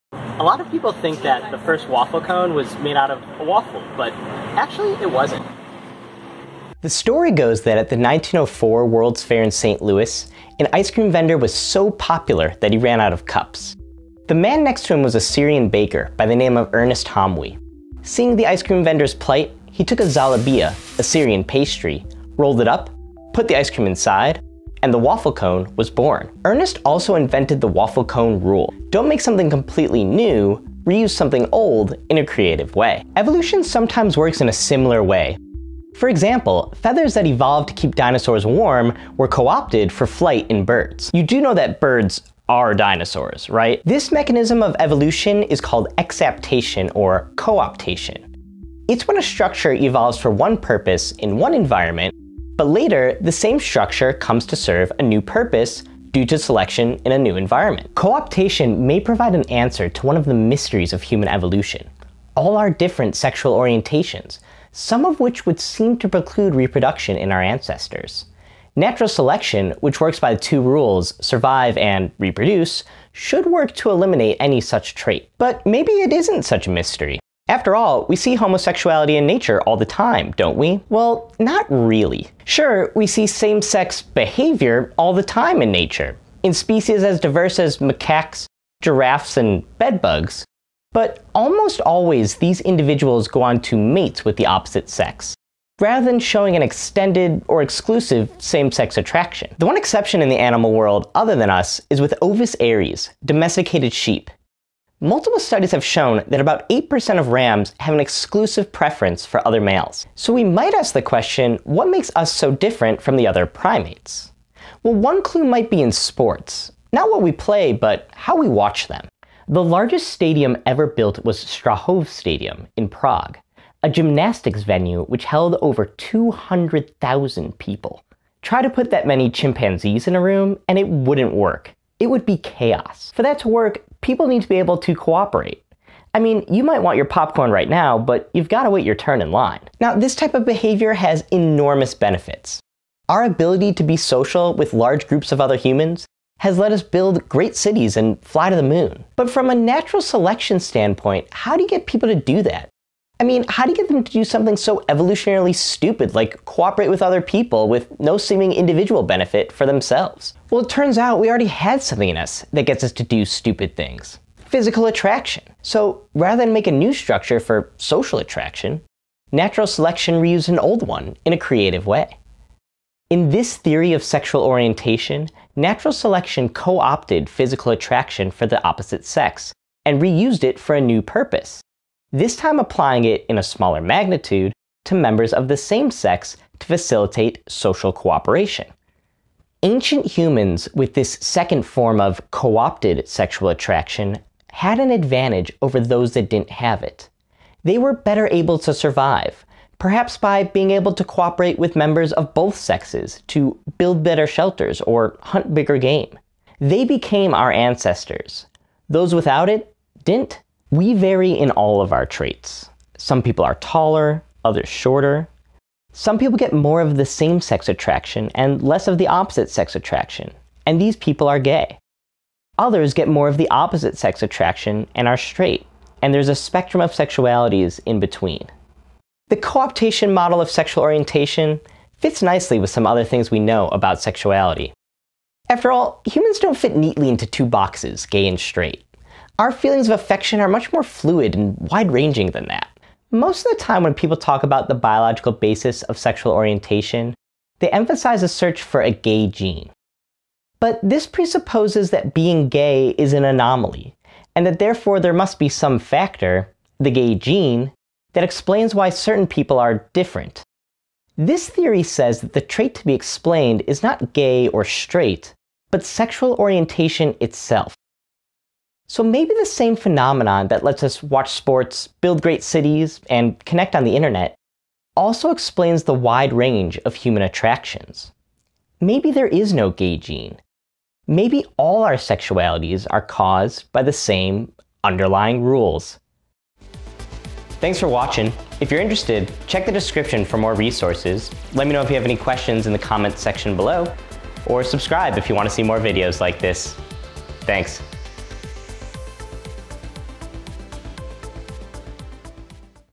How Did Sexuality Evolve?
"The world is not to be divided into sheep and goats...Only the human mind invents categories and tries to force facts into separated pigeon-holes."
-Alfred Kinsey

-I first came across the "socio-sexual" theory of sexuality in a paper by Anthony Caswell. He also writes about it in his book "The Social Gene"

-See this article for other theories on the evolution of sexuality

CREDITS:

Photo of female macaques by Paul Vasey.

Other images used in the public domain.

I thought about calling this video "Is there a gay gene?" but then I realized that's not what it's about.